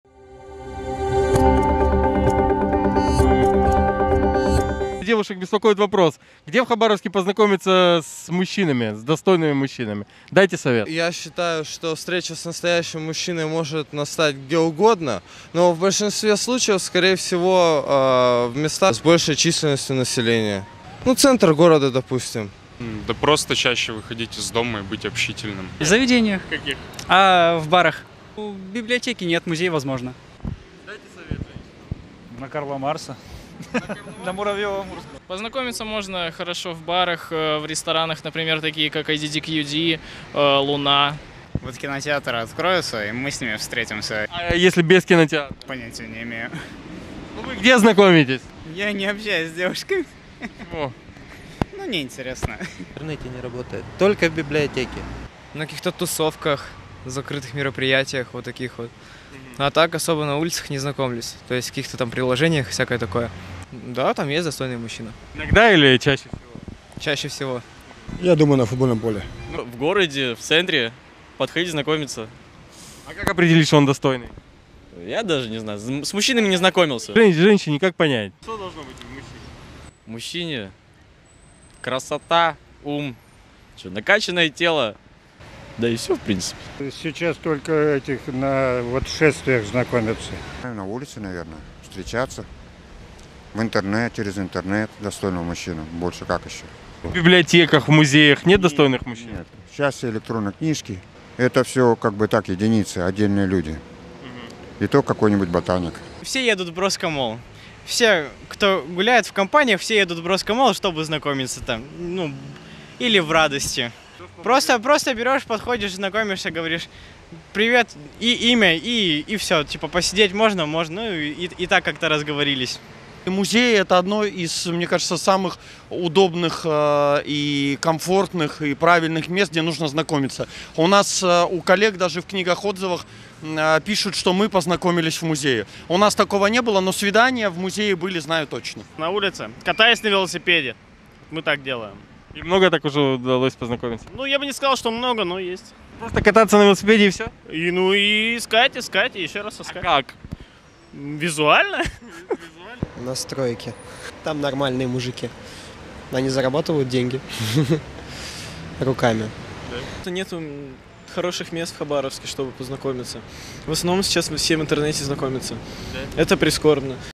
Где в Хабаровске девушкам познакомиться с достойным мужчиной (ВИДЕОБЛИЦ)
Тысячи хабаровчанок задаются вопросом, где в Хабаровске познакомиться с достойным молодым человеком, с которым можно прожить счастливую жизнь. Мужчины попытались дать совет. Что из этого получилось, вы узнаете из нашего традиционного видеоблица.